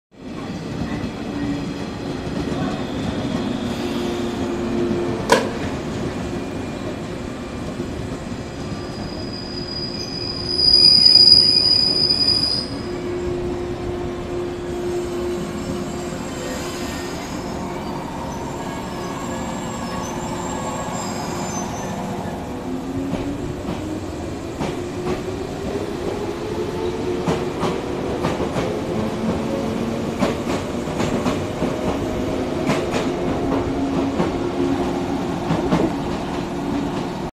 JR伯備線 普通列車115系備中高梁行 ｵｶG-03編成+ｵｶG-04編成 倉敷駅発車
倉敷駅を発車普通列車備中高梁行です